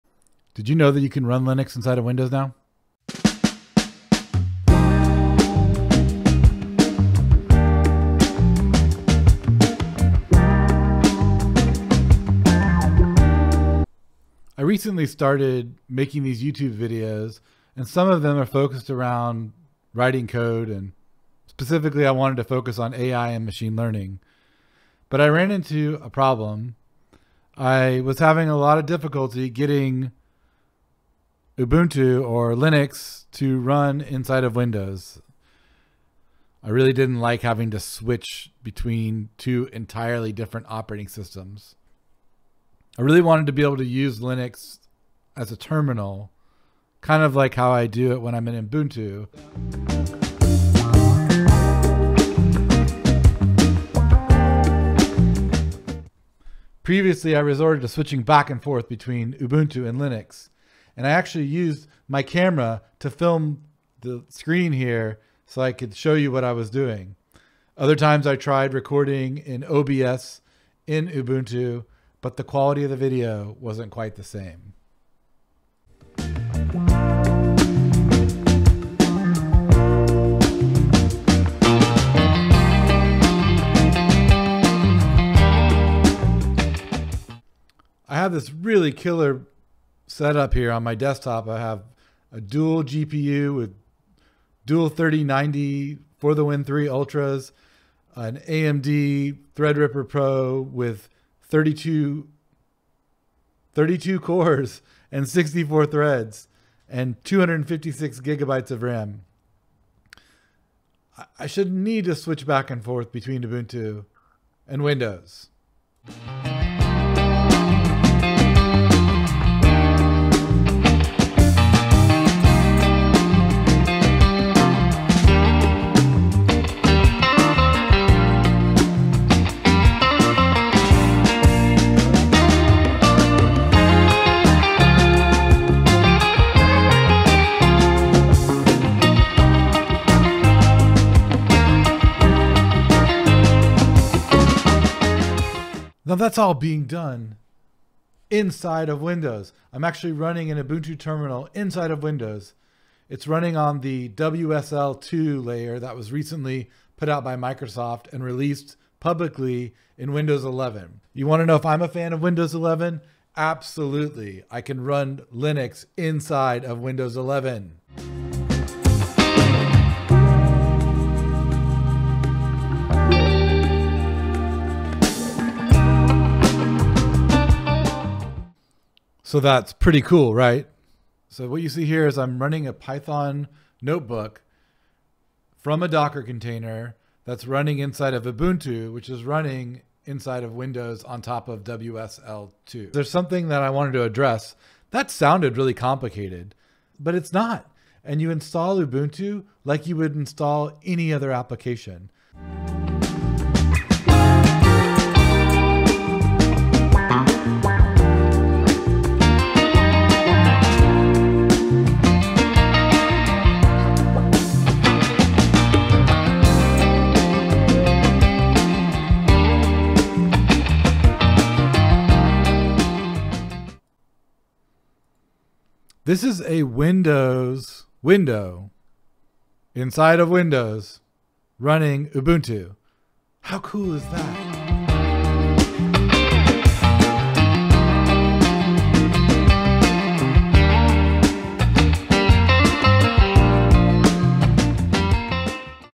windows 11 is 1001% better
I find it very exciting that Windows 11 can now run Linux kernels natively.